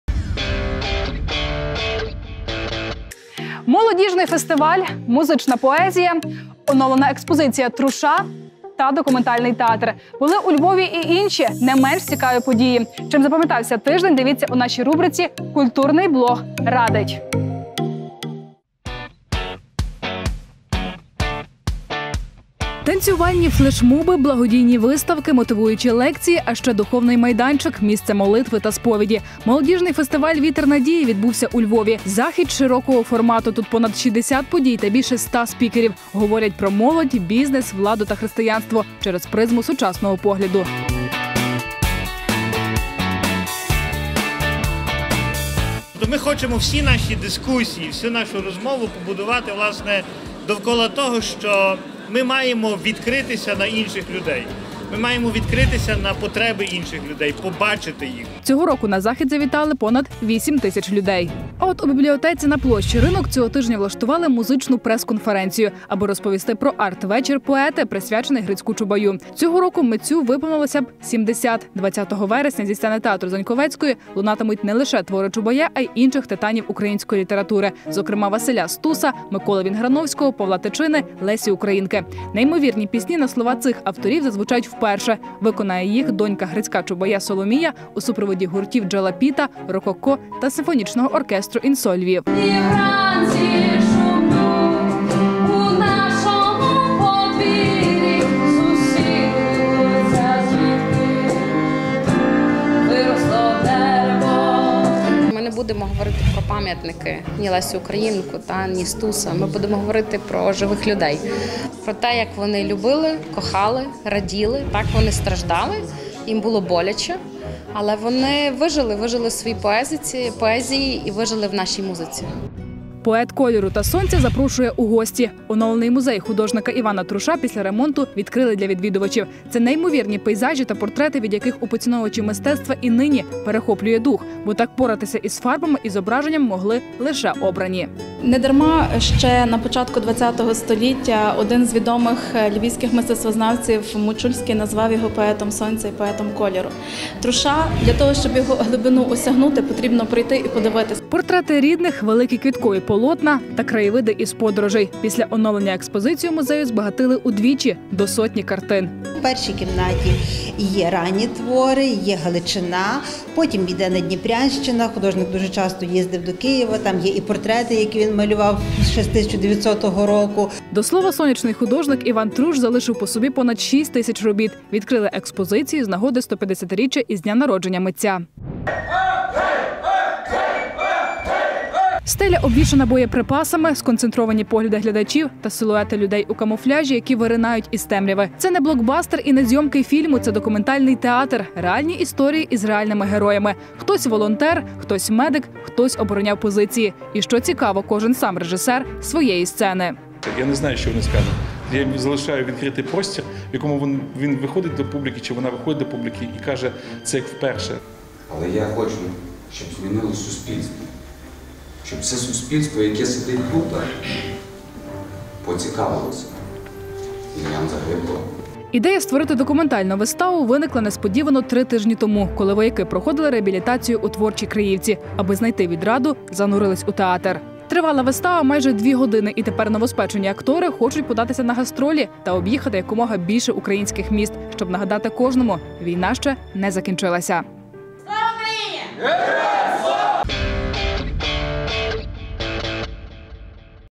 Культурний блог: фестиваль "Вітер На-дії", поезія Чубая, експозиція Труша, вистава "ГОЛОСИ"
Молодіжний фестиваль, музична поезія, оновлена експозиція Труша та документальний театр.  Були у Львові і інші не менш цікаві події. Чим запам'ятався тиждень  - дивіться у нашій рубриці "Культурний блог радить"